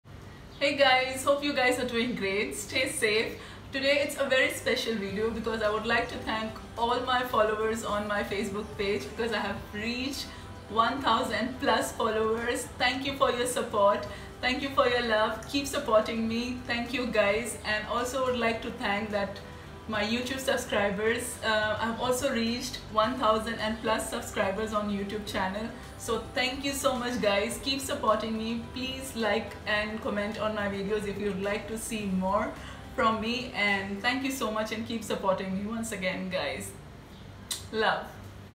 1000 Subscribers On Youtube | Unzee Food And Route
Big Thanks to All of You for the Support 🙏🙏🙏Unzee Food And Route has reached 1000 Subscribers and Many more to come. Keep supporting me guyz Thanks Loads of love to all my Subscribers. ❤️😘🥰🥰